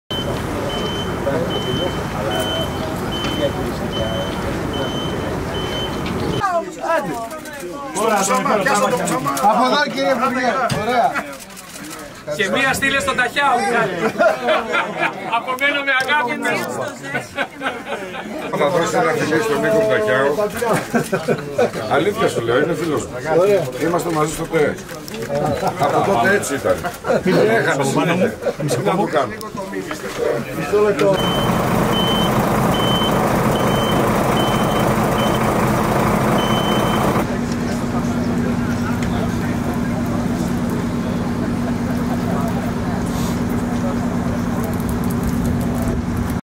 "Στείλε μια φωτογραφία στον Ταχιάο" - Σπίρτζης "Δώστου ένα φιλί,από παλιά έχανε συνέχεια"
Αιχμές... προς τον υποψήφιο δήμαρχο Θεσσαλονίκης με τη Νέα Δημοκρατία, Νίκο Ταχιάο,  από τον πρόεδρο της Αττικό μετρό κ. Μυλόπουλο και τον υπουργό Υποδομών και Μεταφορών, Χρήστο Σπίρτζης την ώρα που οι πρώτοι συρμοί πατούσαν στις ράγες του μετρό στη Θεσσαλονίκητην άφησε .

 "Στείλε μια φωτογραφία στον Ταχιάο" είπε ο κ. Μυλόπουλος απευθυνόμενος προς τους δημοσιογράφους που παραβρέθηκαν στο αμαξοστάσιο της Πυλαίας, ενώ ο υπουργός συμπλήρωσε "Να δώσεις ένα φιλί στον φίλο μου τον Ταχιάο" και συνέχισε: "Ήταν φίλος μου, ήμασταν μαζί στο Τεχνικό Επιμελητήριο Ελλάδας. Από τότε έτσι ήτανε.. έχανε συνέχεια, τι να τον κάνω" είπε, υπονοώντας ότι ο Νίκος Ταχιάος θα χάσει την εκλογική μάχη για το δήμο Θεσσαλονίκης.